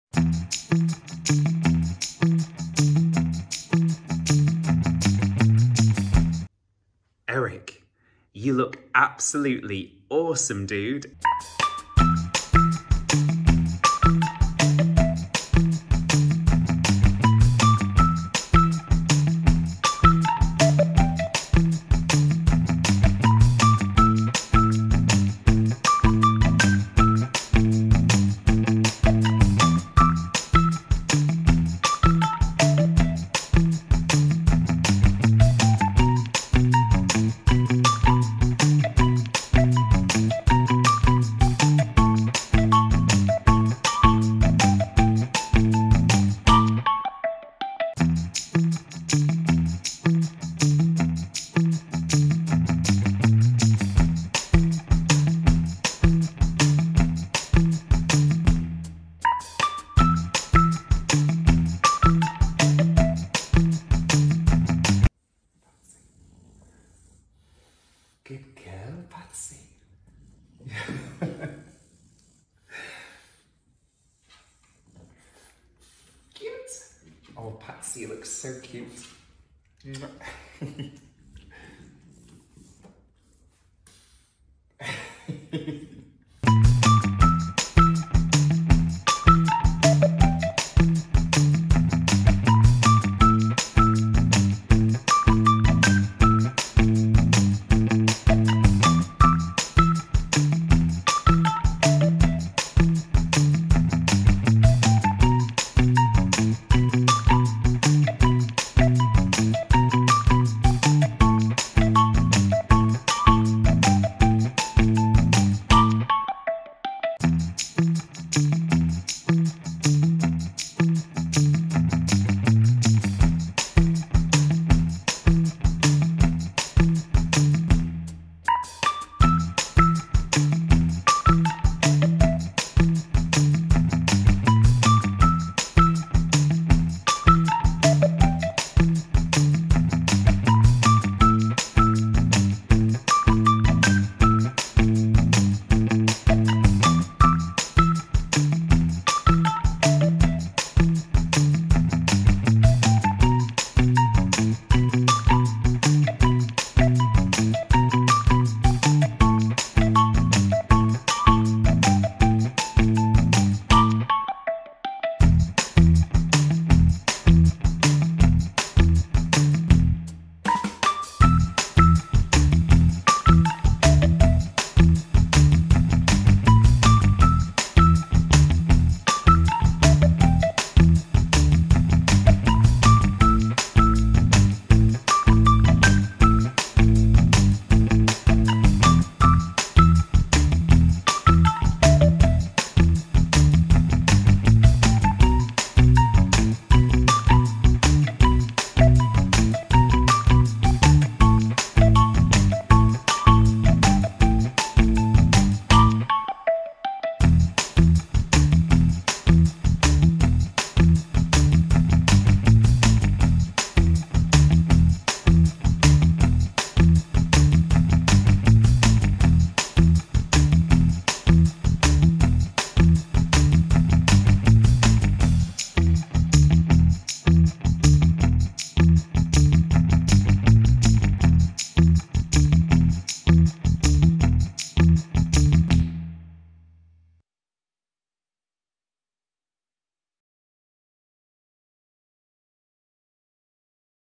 French Bulldog TikToks (Behind The Scenes part 3!)
Join French Bulldogs Eric Charming & Patsy Darling for more exclusive behind the scenes action! We've just raised our Wotsit game & reached 20k followers on TikTok...follow us @eric_charming